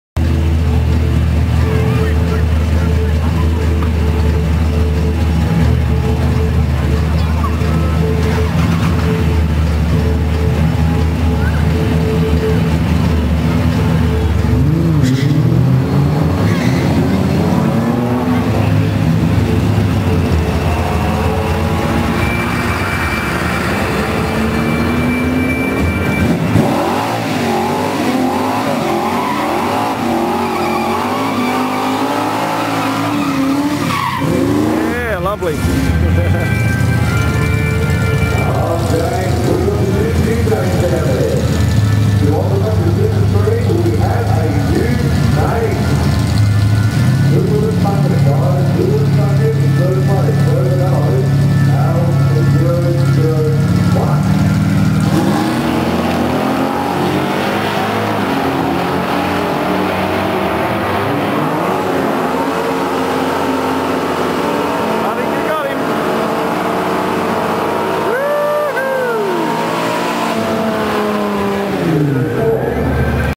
charger 11.5 run
Charger 360 vortech efi , Kwinana motorplex oct 2006